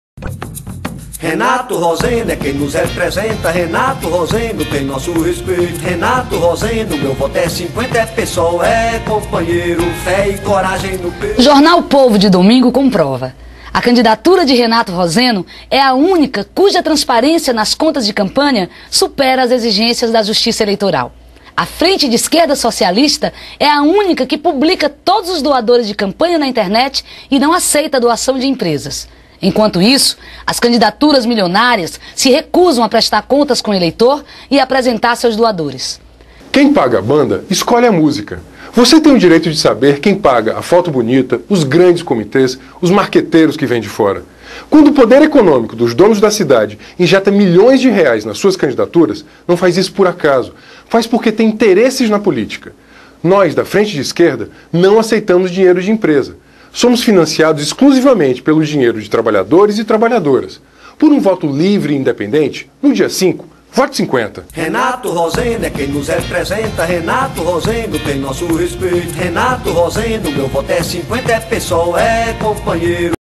Renato Roseno Prefeito 50 Fortaleza
Programa eleitoral da campanha Renato Roseno 50 Prefeito Fortaleza - Frente de Esquerda (PSol - PSTU), exibido em 10/09/2008